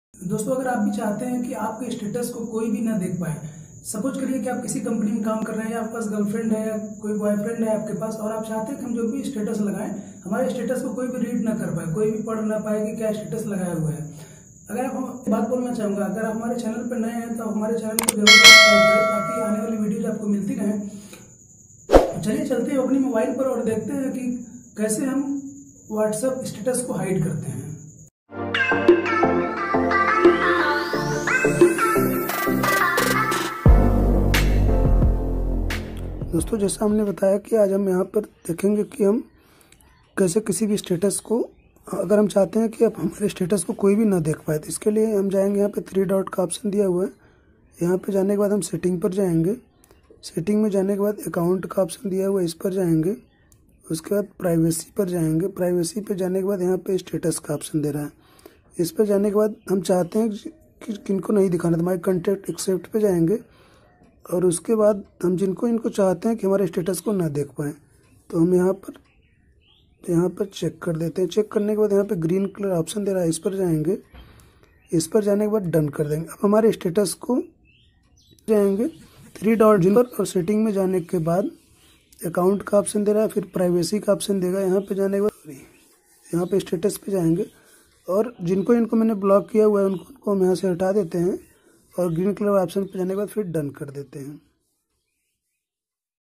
How to hide whats app status / अपना वाट्सएप स्टेट्स छुपाए
is video me aap sikhenge.
  ki kiase apna whatsapp status ko kaise chupaaye
how to hide whatsapp status
how to unhide whatsapp status
whatsapp trick -whatsapp status ko kaise chupaye
Conncet us on social media.